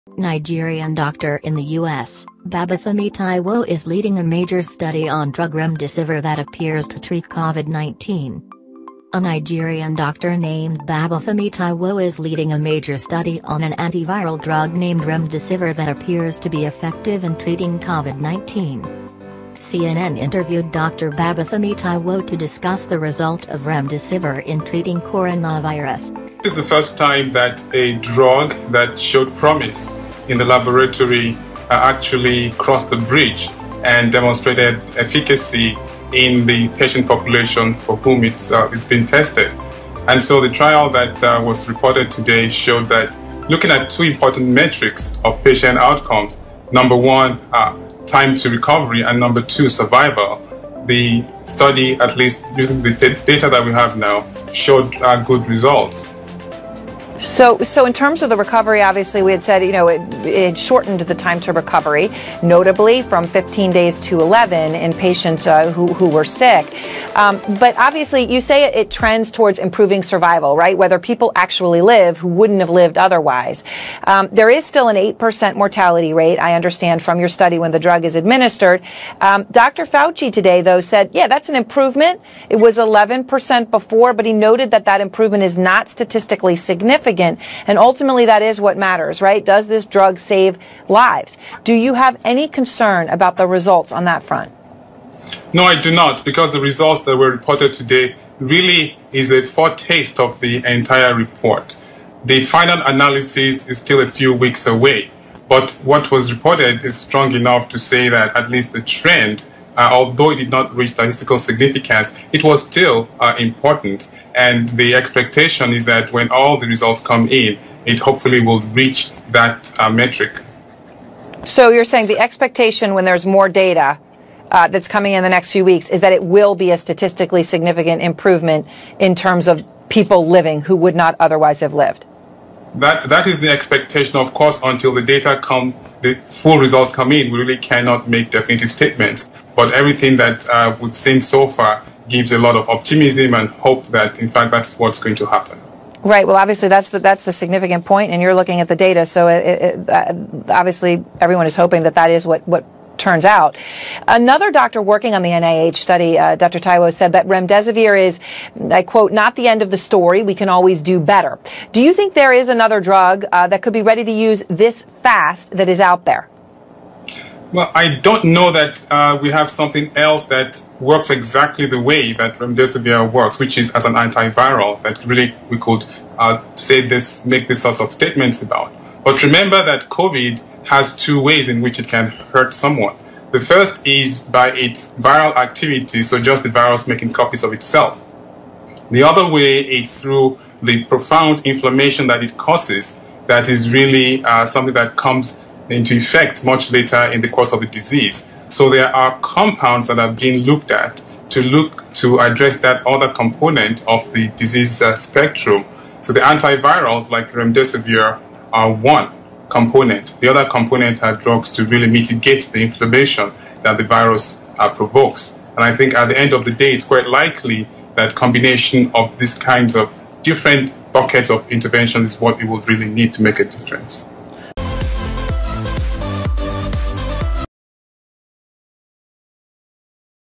Nigerian doctor in US, is leading major study on drug Remdesivir that appears to treat COVID-19
Nigerian doctor in US, Babafemi Taiwo is leading major study on drug Remdesivir that appears to treat COVID-19

A Nigerian doctor named Babafemi Taiwo is leading a major study on an antiviral drug named Remdesivir that appears to be effective in treating COVID-19.

CNN interviewed Dr Babafemi Taiwo to discuss the result of Remdesivir in treating Coronavirus.

Preliminary results of Remdesivir show it can help hospitalized COVID-19 patients recover faster.

Though Remdesivir, an antiviral drug made by Gilead Sciences, appears to help patients with Coronavirus recover faster, it does not significantly reduce death, according to preliminary data from the National Institutes of Health.

"The data shows that Remdesivir has a clear cut, significant, positive effect in diminishing,  the time of recovery," the director of the National Institute of Allergy and Infectious Diseases, Dr Anthony Fauci said during a meeting between president Trump and Louisiana Governor John Bel Edwards.

Below is a video of Dr Babafemi Taiwo speaking to CNN about the promising results of Remdesivir in treating COVID-19.